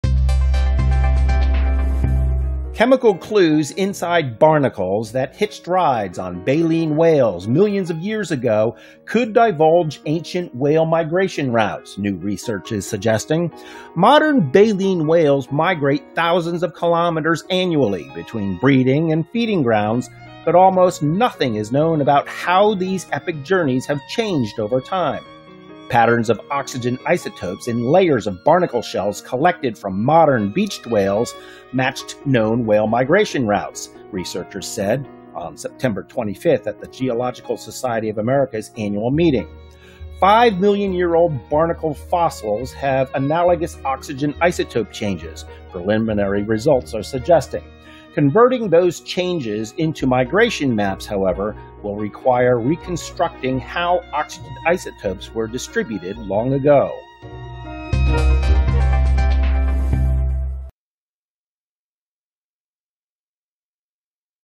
Barnacles track whale migration
Chemical clues inside barnacles that hitched rides on baleen whales millions of years ago could divulge ancient whale migration routes, new research suggests.
Modern baleen whales migrate thousands of kilometers annually between breeding and feeding grounds, but almost nothing is known about how these epic journeys have changed over time. Patterns of oxygen isotopes in layers of barnacle shells collected from modern beached whales matched known whale migration routes, Taylor said September 25 at the Geological Society of America’s annual meeting. Five-million-year-old barnacle fossils have analogous oxygen isotope changes, preliminary results suggest. Converting those changes into migration maps, however, will require reconstructing how oxygen isotopes were distributed long ago.